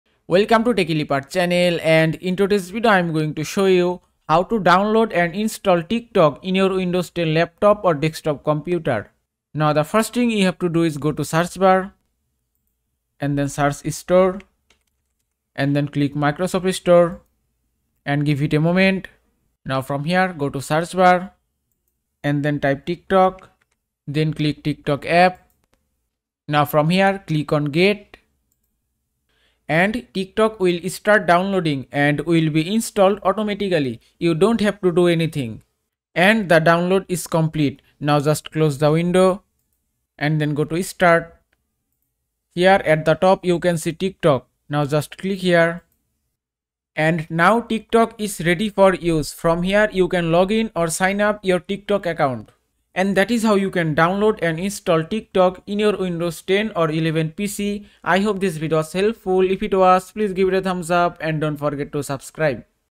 How to Download and Install TikTok in Laptop or PC
A quick guide to download and install Tiktok app in your Windows 10 or 11 laptop or PC.